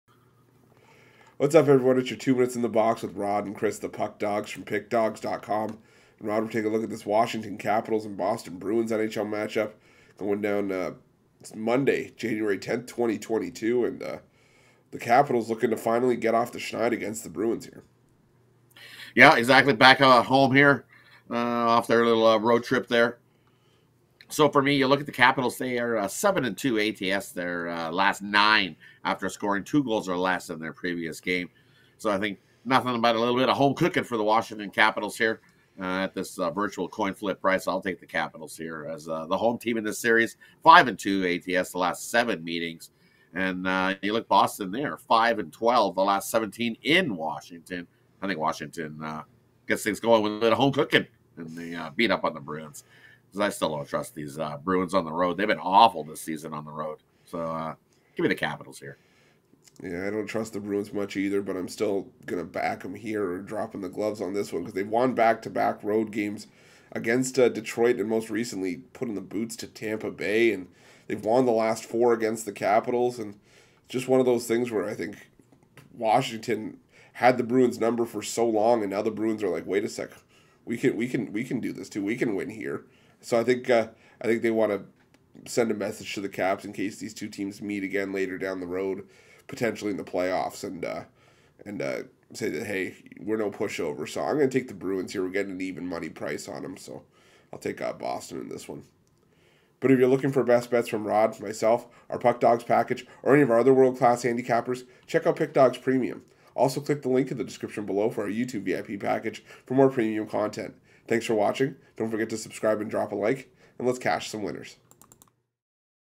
Washington Capitals vs Boston Bruins 1/10/22 NHL Free Pick Free NHL Betting Tips
The Boston Bruins face the Washington Capitals in Monday's NHL Action.